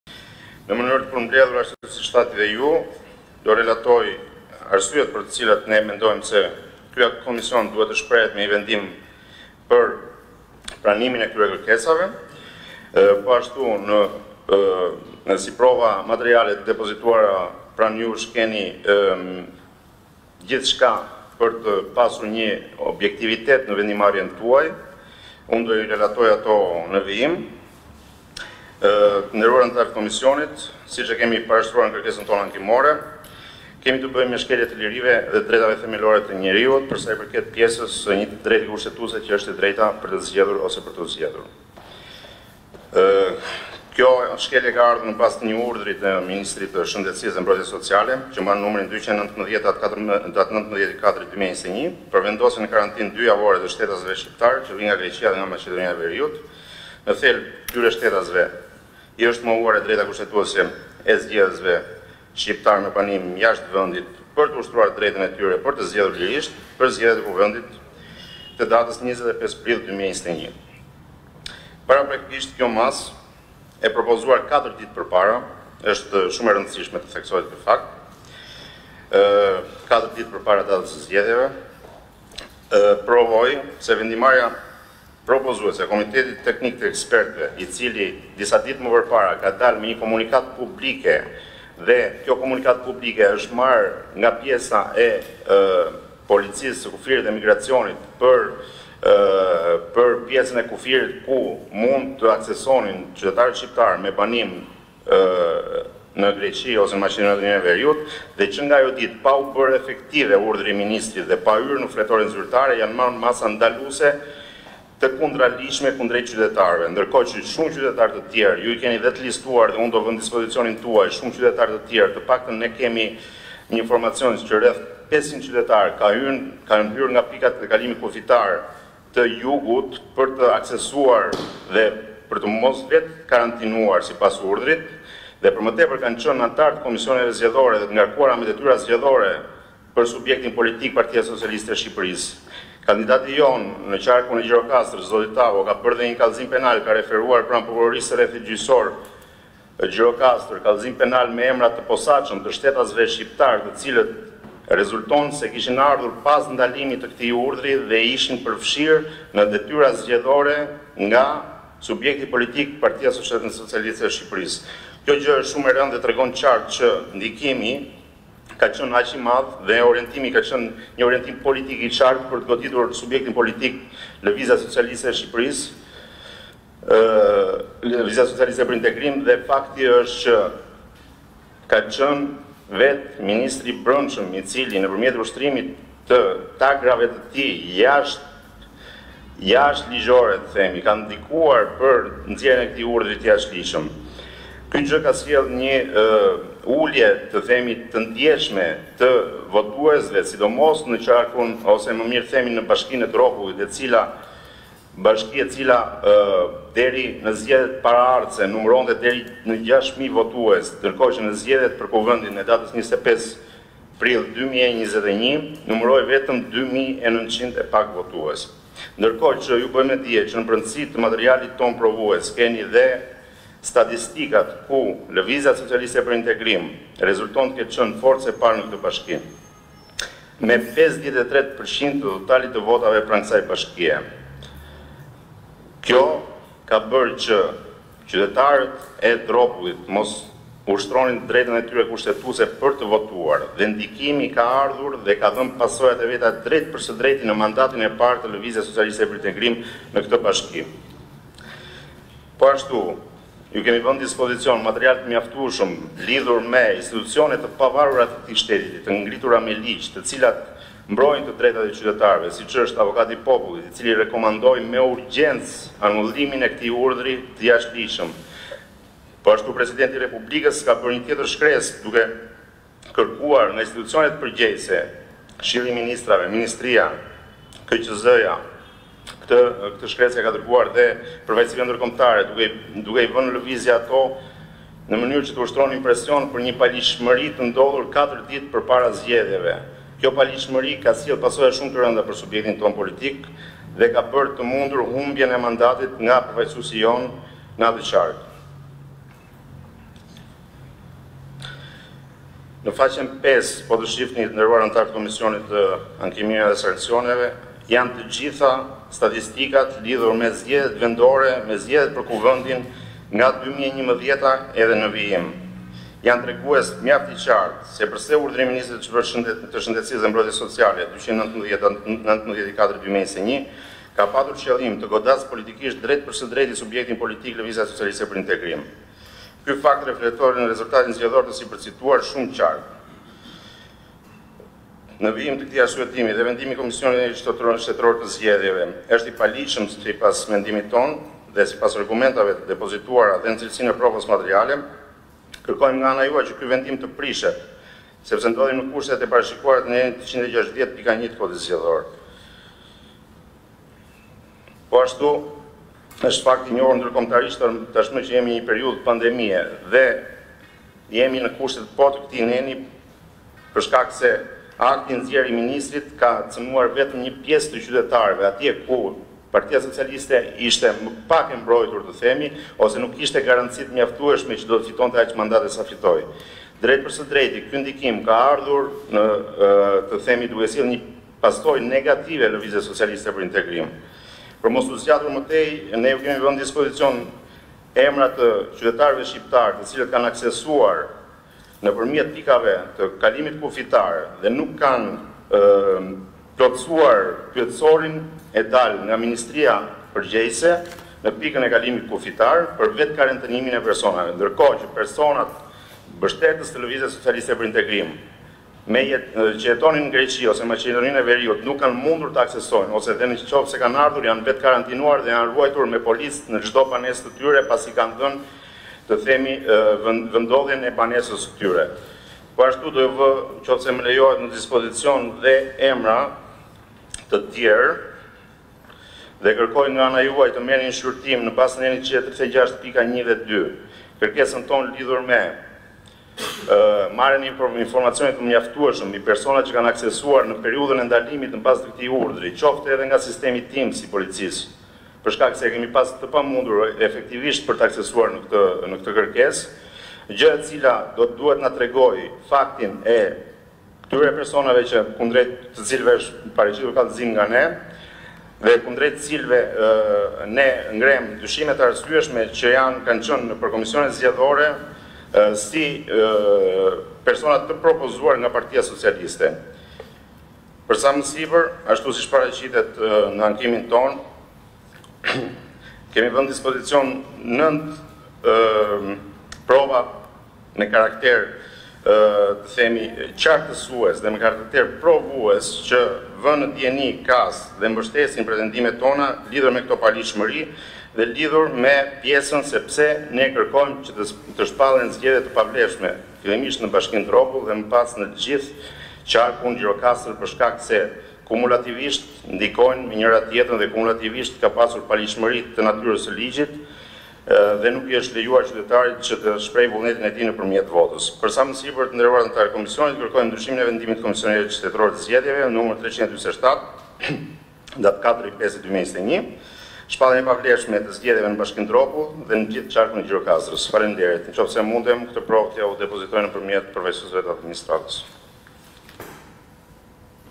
Ankimimi për zgjedhjet në Gjirokastër, LSI: Karantinimi i të ardhurve nga Greqia ndikoi rezultatin
KAS është duke diskutuar në mbledhjen e sotme ankimin e LSI-së për rezultatin e Gjirokastrës pasi ajo pretendon se vendimi i Komitetit Teknik të Ekspertëve për ta karantinuar për 14 ditë qytetarët që vijnë nga Greqia dhe Maqedonia e Veriut, ka synuar që ta godasë politikisht atë në zgjedhjet e 25 prillit.

LSI kërkon që të shpallen të pavlefshme zgjedhjet në Bashkinë Dropull, gjë që ndikon në rezultatin e shpërndarjes së mandateve për Qarkun e Gjirokastrës.

Përfaqësuesi i LSI është shprehur në seancë se edhe ato që erdhën nga Greqia apo Maqedonia e Veriut u ruajtën me policë pasi u ishte marrë adresa e shtëpisë.

Qytetarët e Dropullit nuk e kanë ushtruar të drejtën e tyre për të votuar.  Në ankimimin, LSI sqaron KAS se urdhri për 'vetëkarantinimin e detyrueshëm', për një periudhë dy javore për të gjithë shtetasit që hyjnë në Shqipëri nga Maqedonia e Veriut dhe nga Greqia ka ndikuar drejtpërdrejt në pjesëmarrjen dhe rezultatin e votimit në zonën zgjedhje Qarku Gjirokastër.